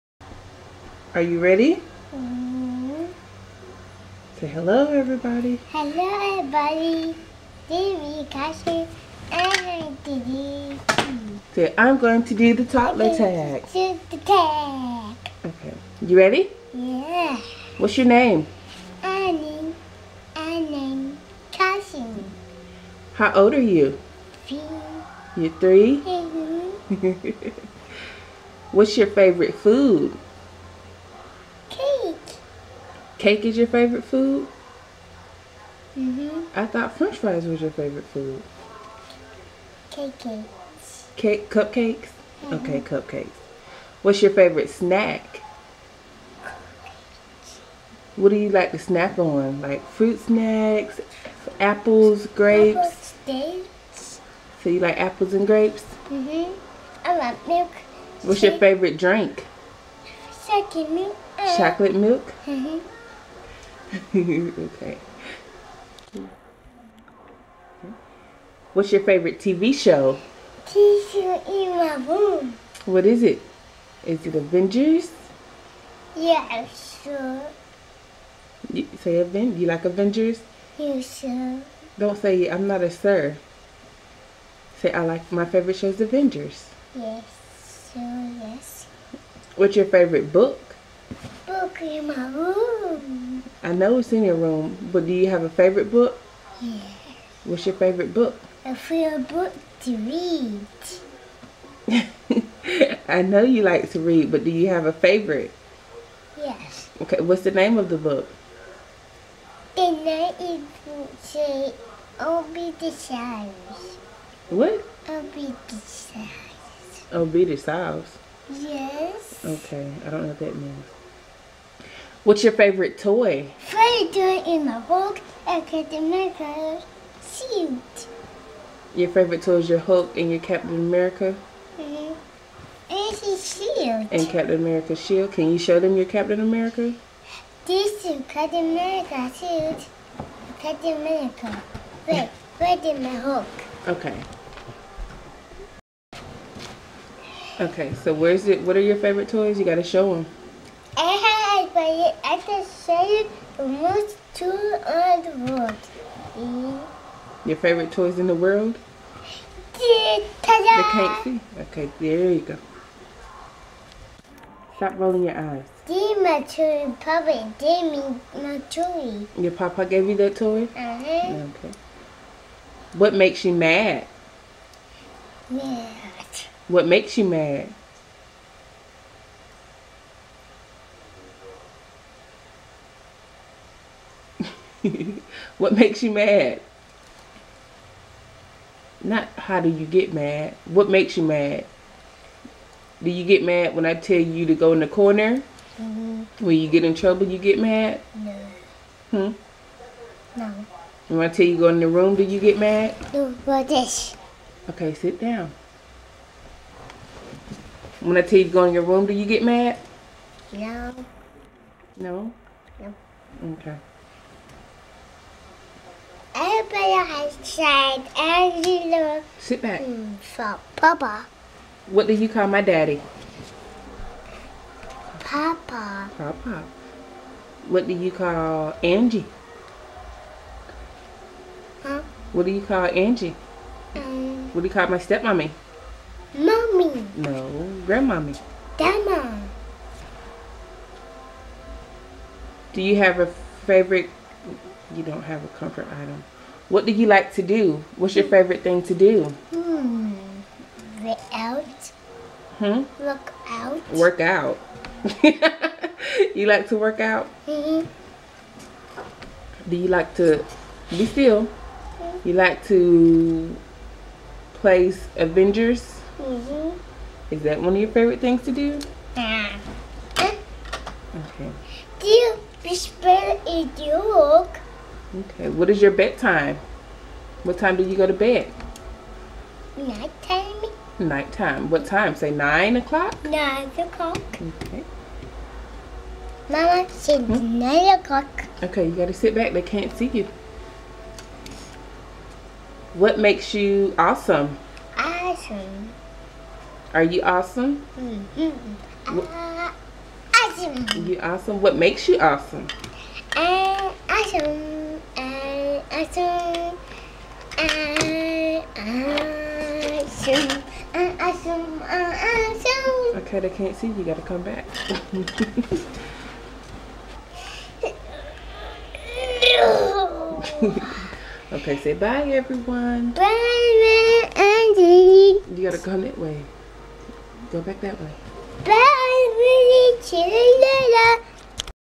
Vlogtober| Day 4 Toddler Tag
Hey ya'll I got this tag from Tiffany from the Daily Davidsons. My son loves to be in front of the camera so I decided to do a video just for him. I hope you all enjoy